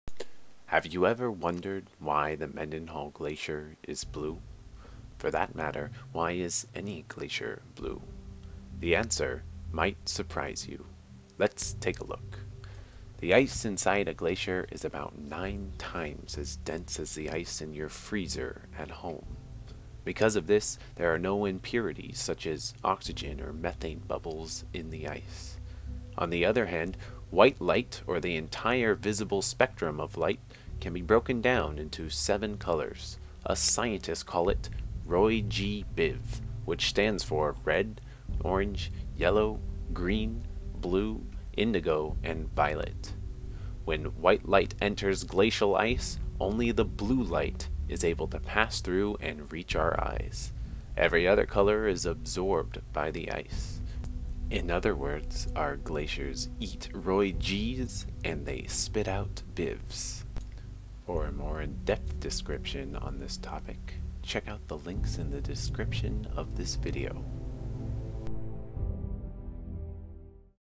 Why Glaciers are Blue
A brief explanation as to why glaciers are blue.